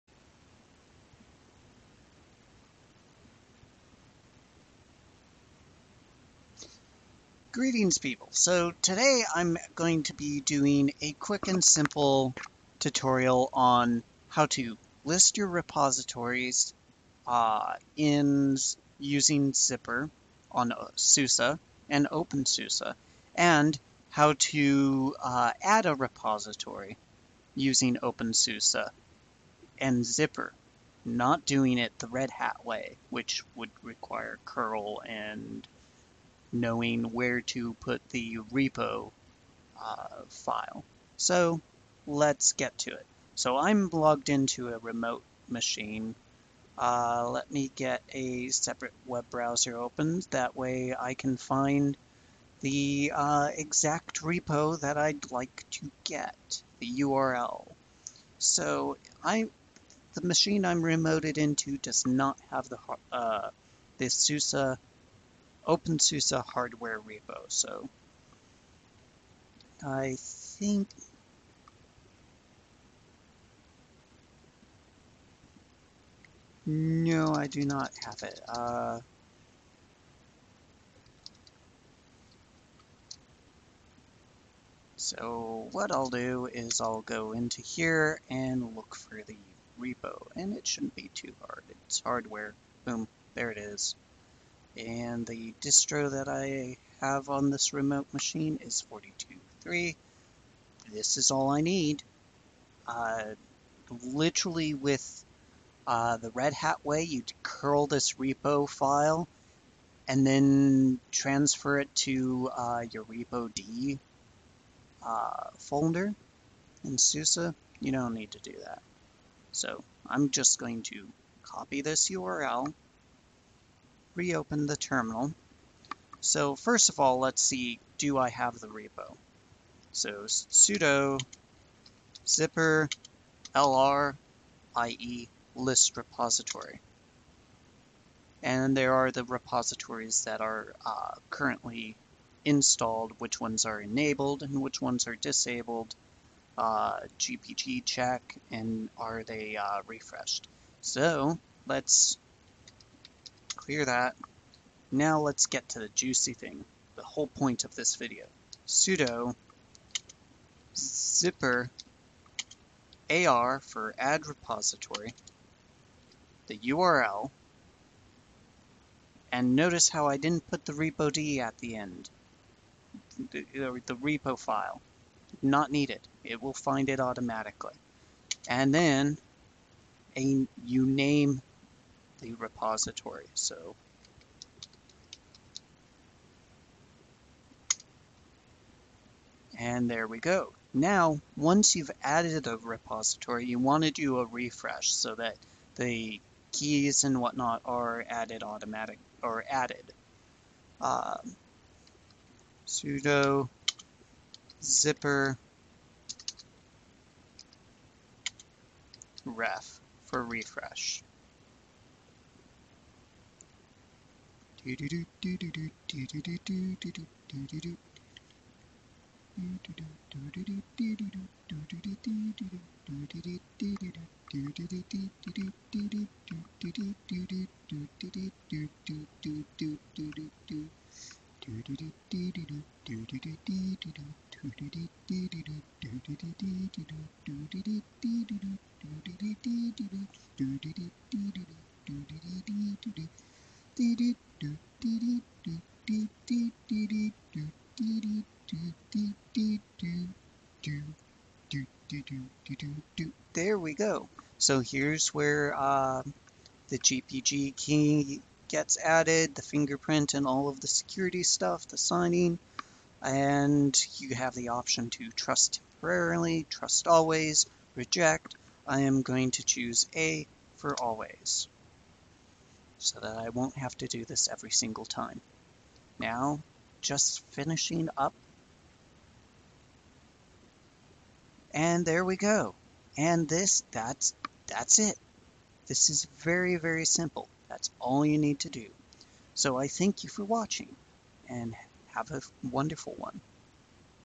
How to add a repo using Zypper in Opensuse/SUSE
In this video you will be learning how to list repositories, how to add a repo and how to refresh your repos all while using zypper.

If you liked this tutorial Video please think about donating at either Any contributions will go a long way in making more and better quality tutorials.